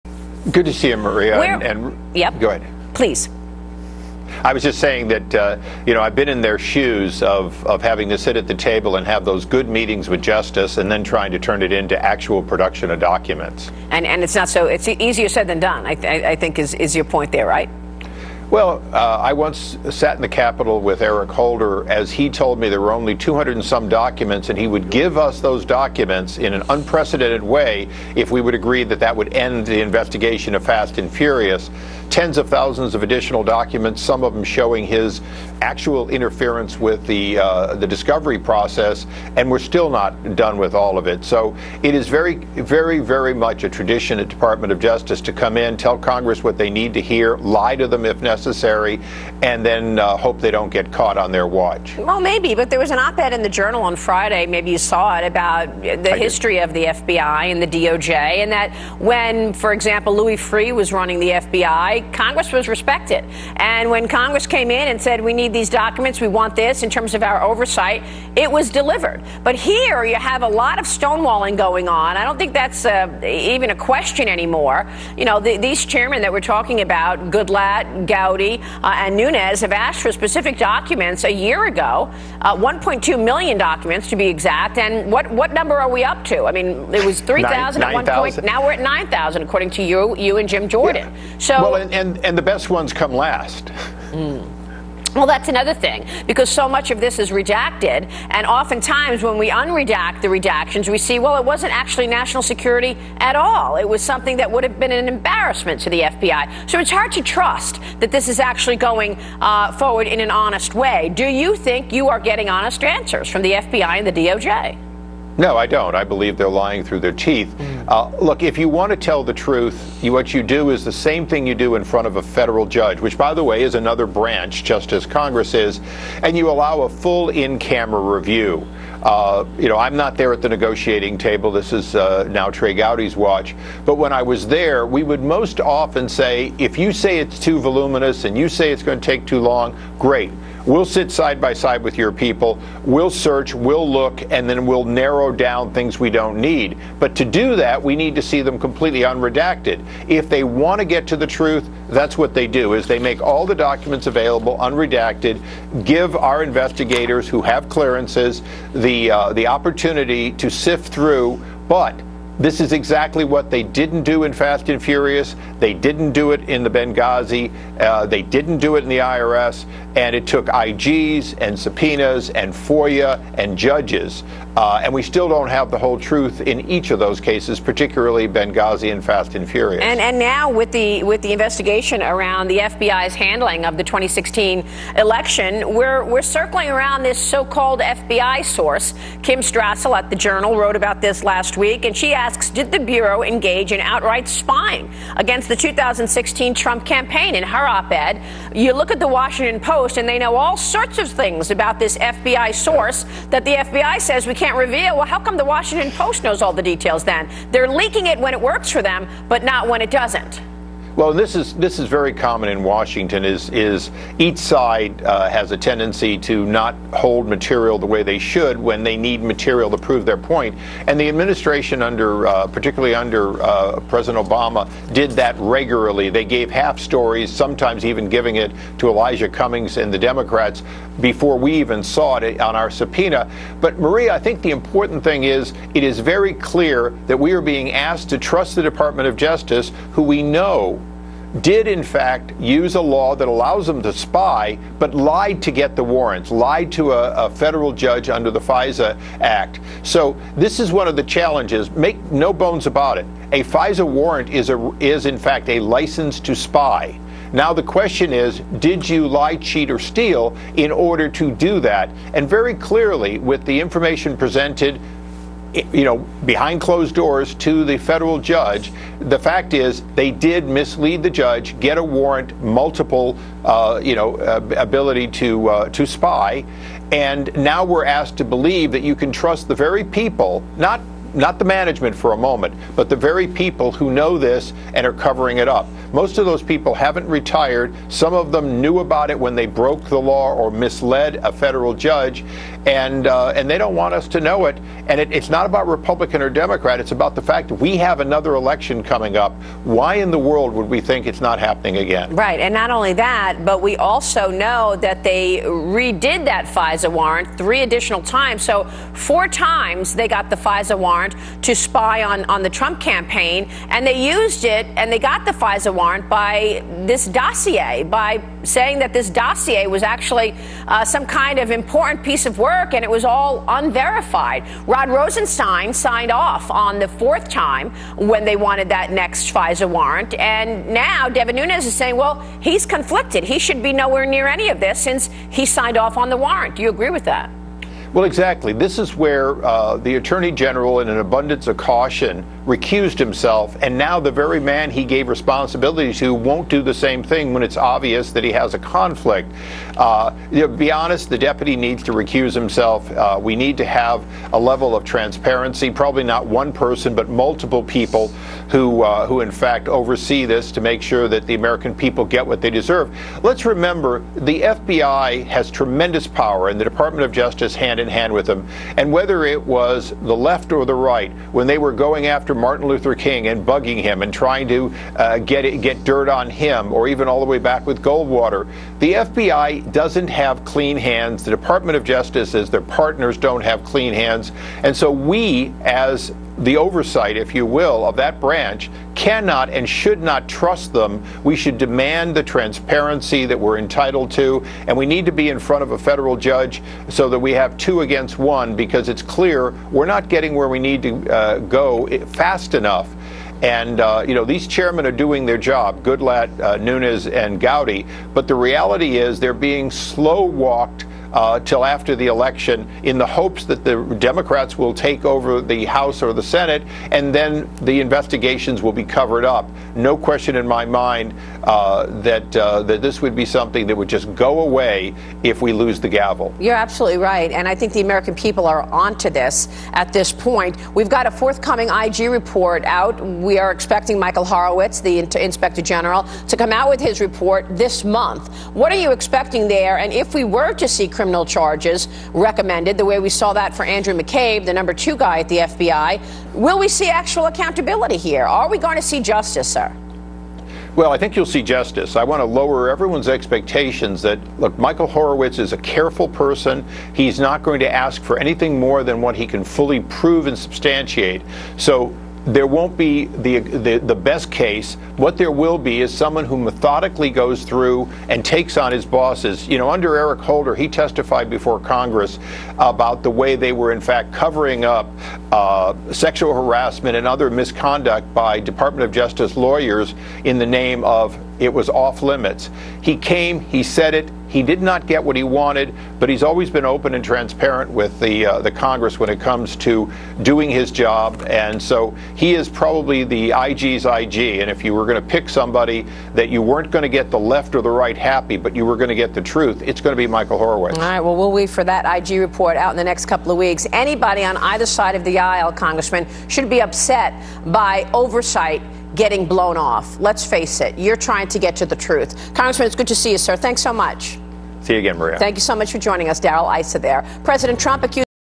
Bombshell Interview: Rep. Darrell Issa Says Deep State Lied to Get FISA Warrants to Spy on Trump
Bombshell Interview: Rep. Darrel Issa Says Deep State Lied to Get FISA Warrants to Spy on Trump